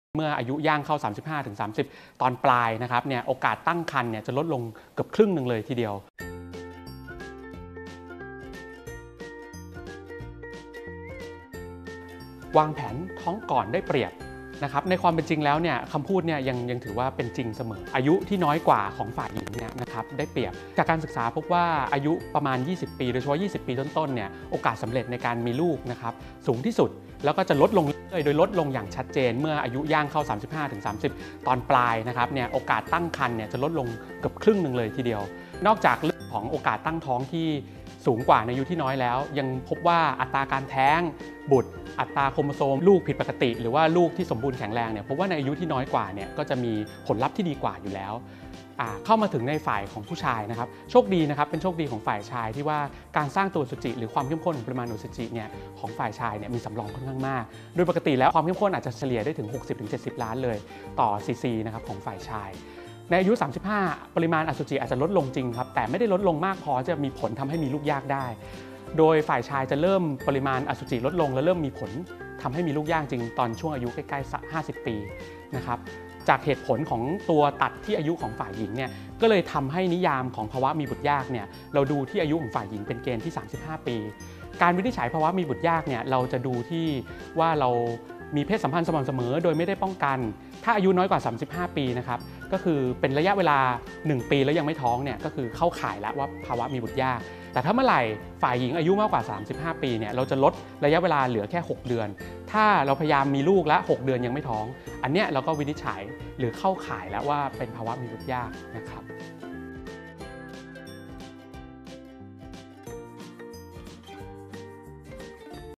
อยากตั้งครรภ์ต้องทำอย่างไร
วางแผนมีลูกทั้งฝ่ายหญิงและฝ่ายชาย❗ ปัจจัยเสี่ยงที่เป็นเรื่องเล็กอาจทำให้เสี่ยงต่อการมีบุตรยาก ❗
โดย นายแพทย์ธนัท จิรโชติชื่นทวีชัย
แพทย์ผู้เชี่ยวชาญทางด้านสูตินรีเวช (เวชศาสตร์การเจริญพันธุ์)